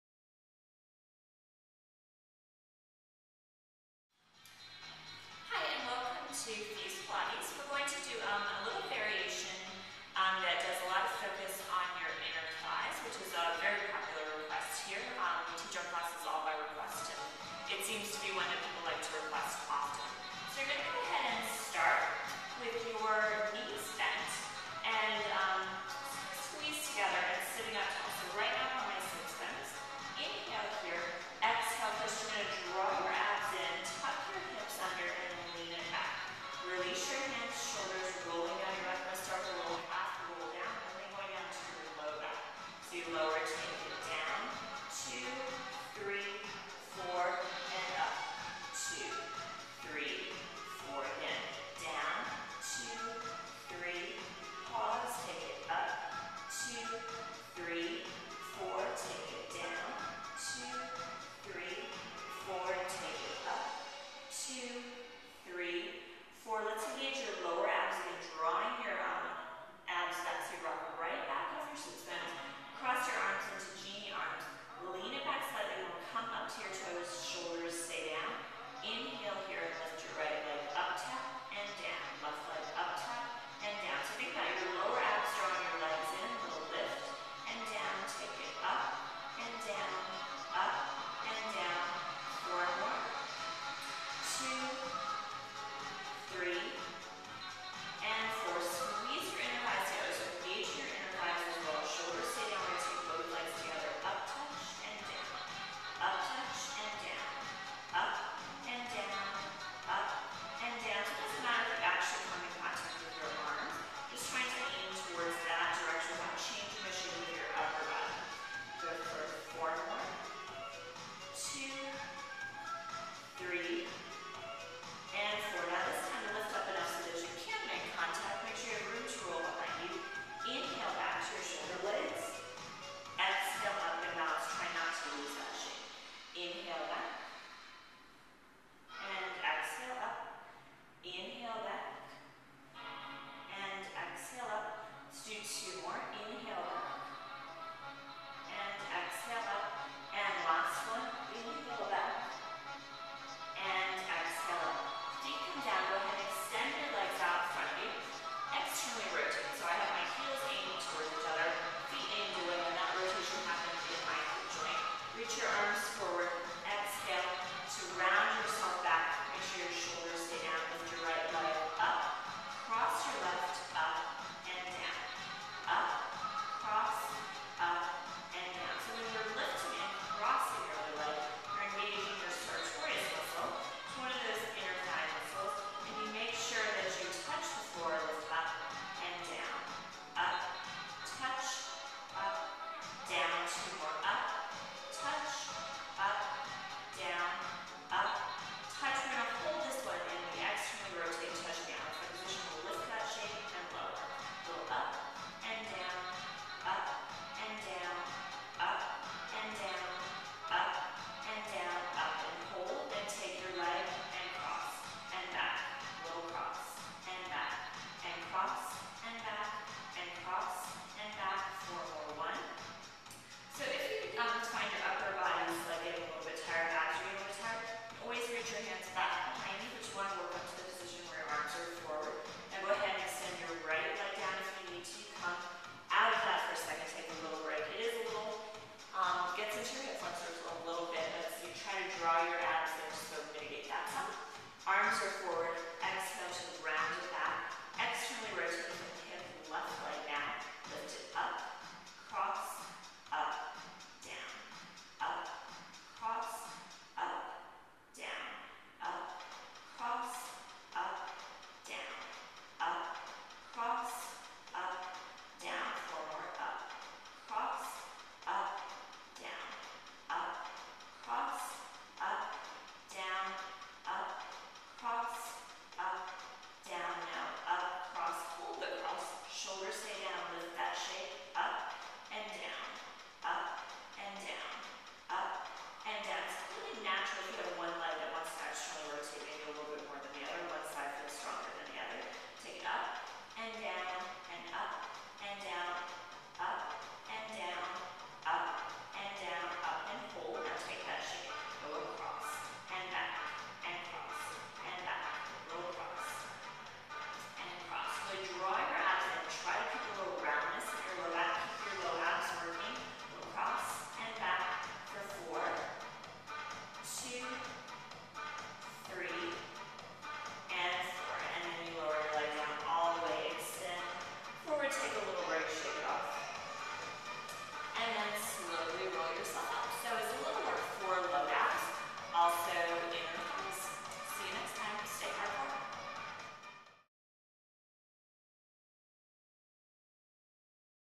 Mini workout - Lower abs and inner thighs
Mariska demonstrates a Fuse Pilates sequence combining lower abs and inner thigh exercises.